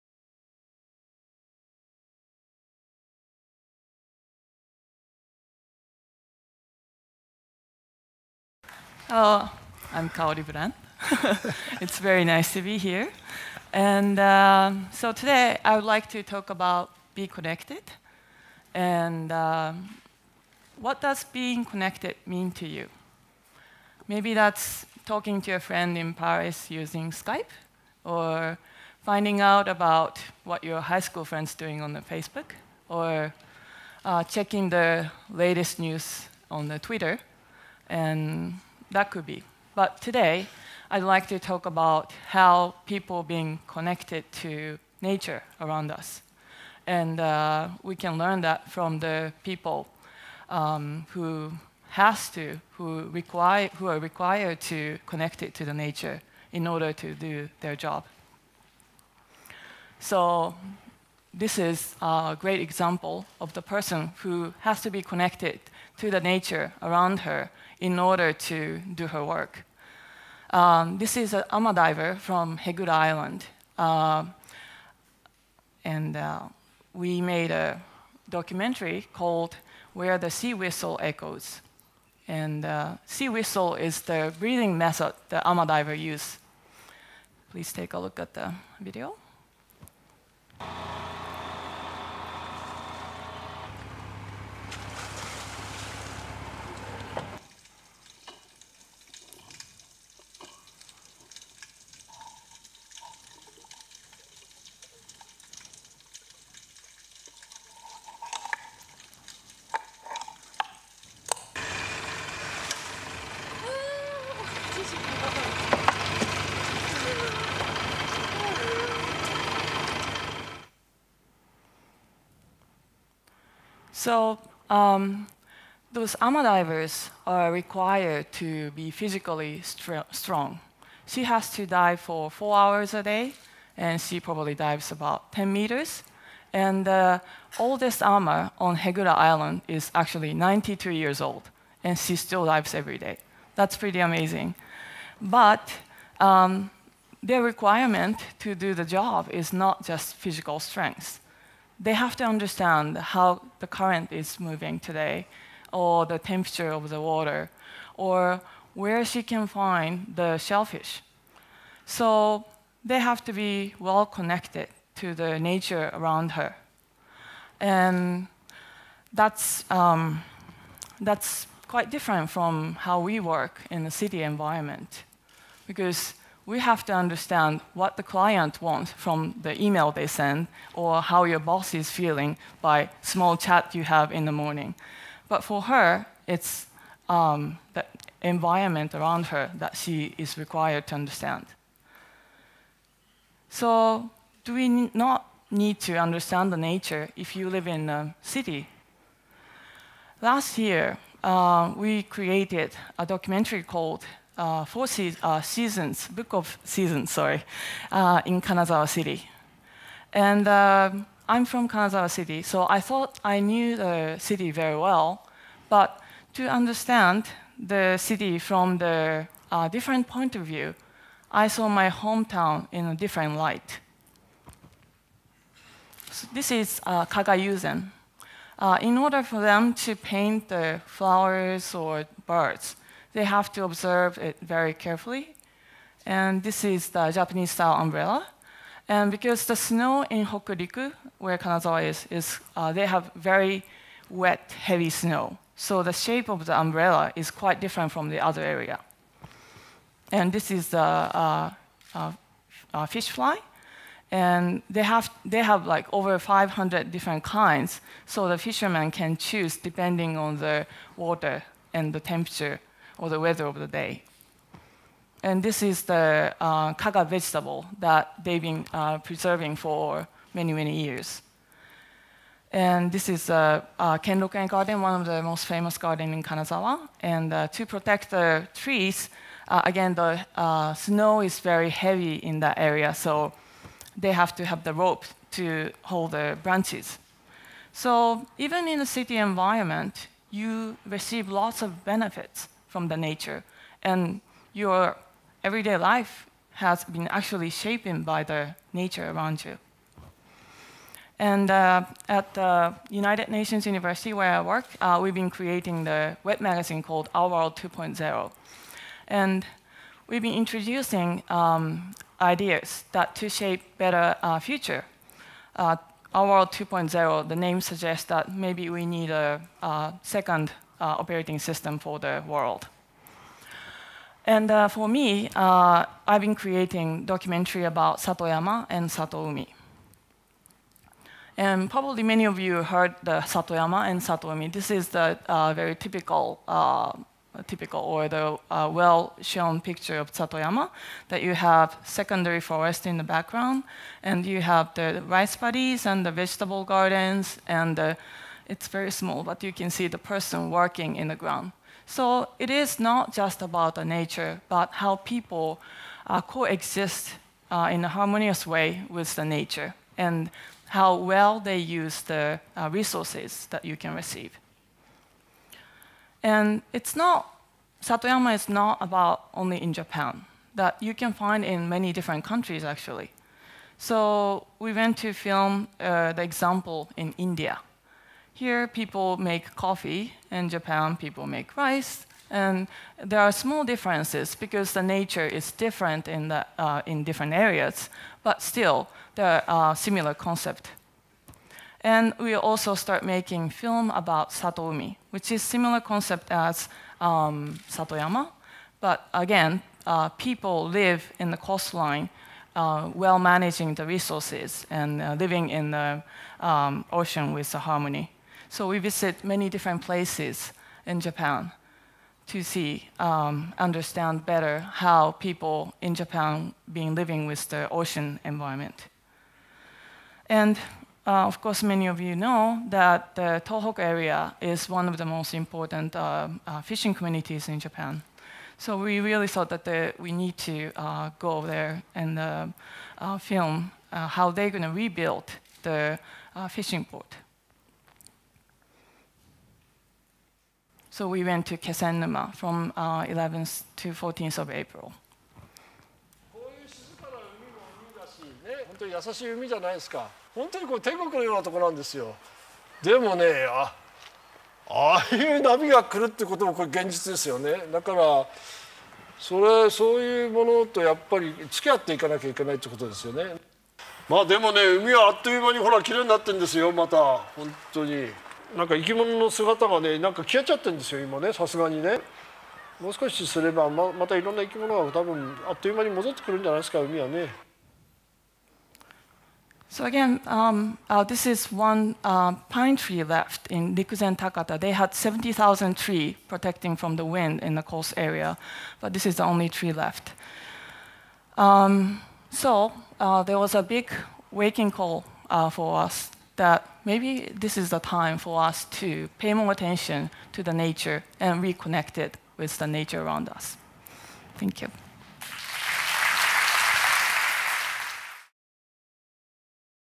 TEDxTokyo - Kaori Brand - Be Connected - [English]
Recorded live at TEDxTokyo on May 21st 2011, at the Miraikan, Odaiba.